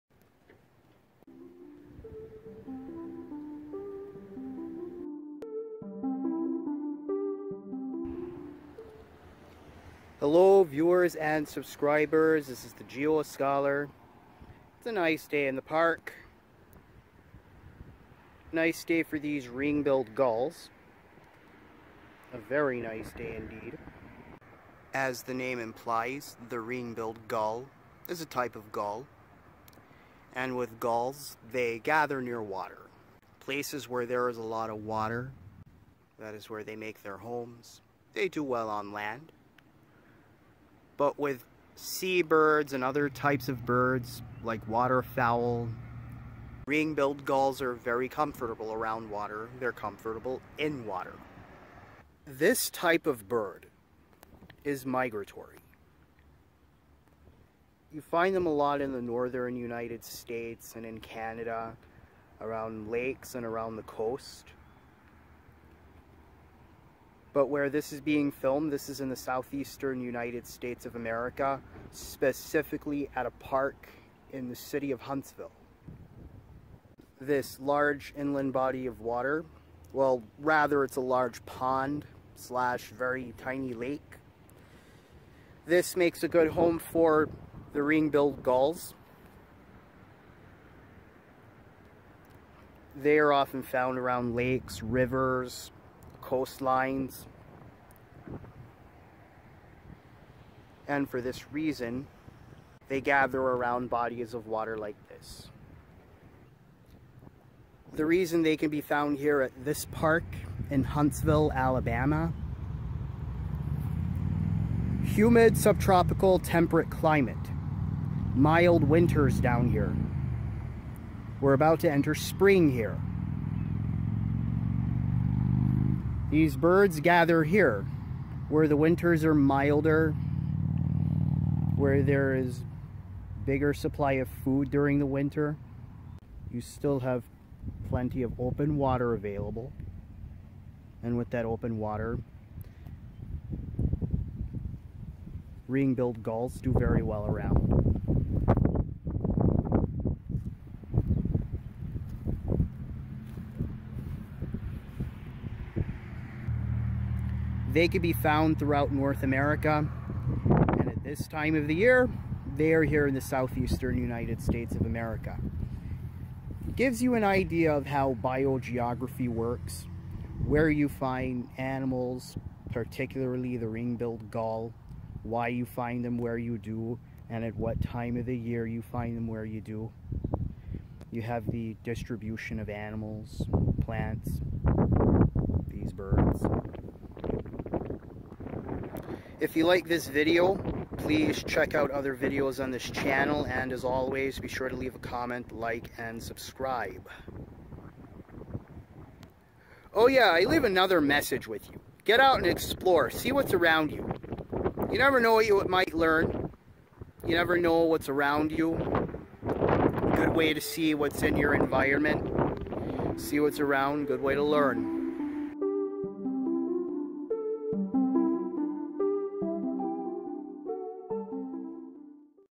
Ring Billed Gull And Biogeography
The ring billed gull is a migratory water bird found in North America.

Follow TheGeoScholar on social media.